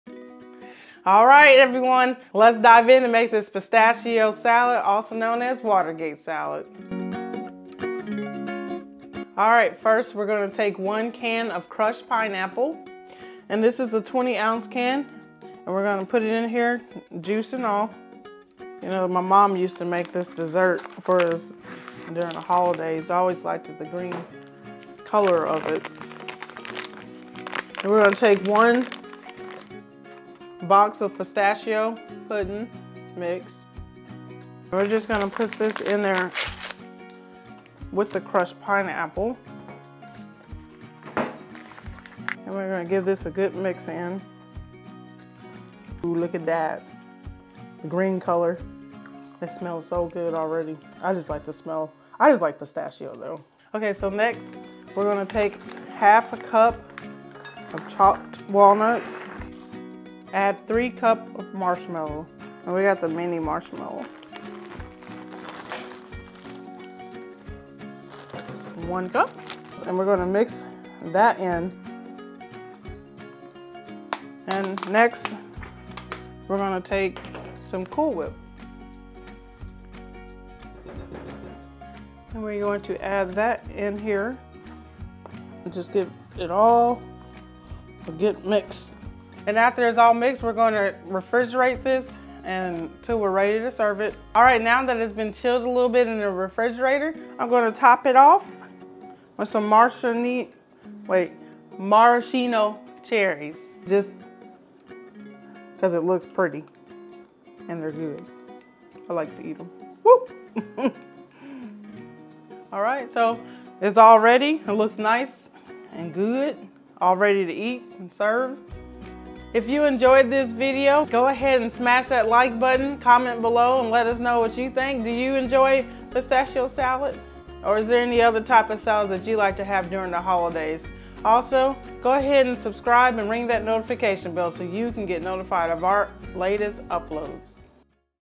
How to make Pistachio Salad (Watergate Salad)
How to make a Quick and Easy Pistachio Salad in under 5 minutes!!